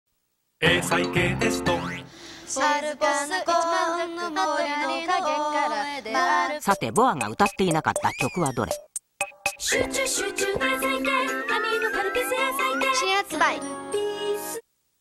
アミノカルピスのＴＶ－ＣＭ（2004年）
2004年3月頃に流れていたCMです。
ＢoAが出演しています。
当時、日曜日のお昼にやっていたフジテレビ「ウチくる！」
の中で放送されていたものです。